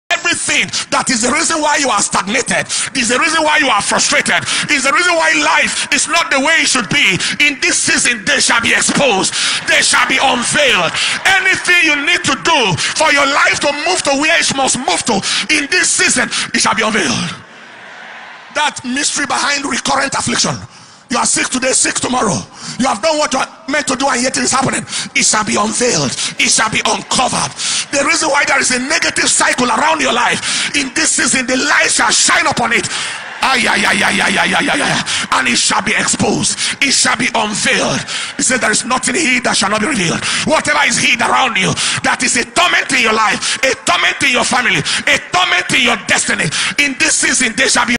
Overcoming Stagnation: Pastor Paul Enenche's Powerful Declarations
Paul Enenche prophetic Declaration Against Stagnation